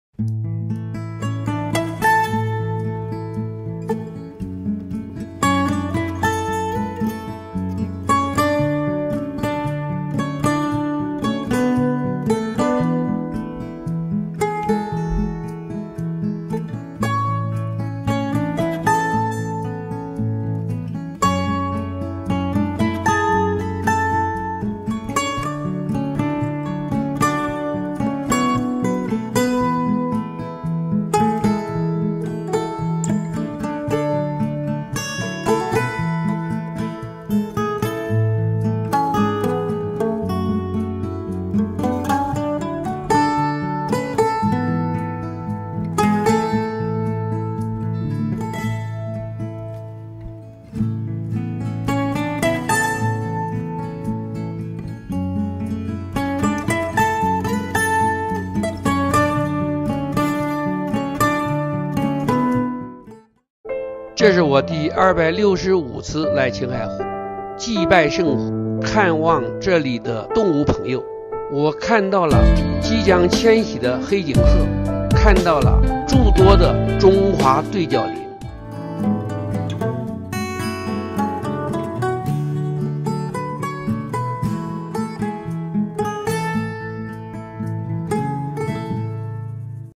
Endangered gazelle population increases around 10 times in 20 years in Northwest China
The Chinese diagonal gazelle is an endangered animal unique to China. After more than 20 years of conservation efforts, the population of the precious species has increased around 10 times, from 300 to more than 2,700.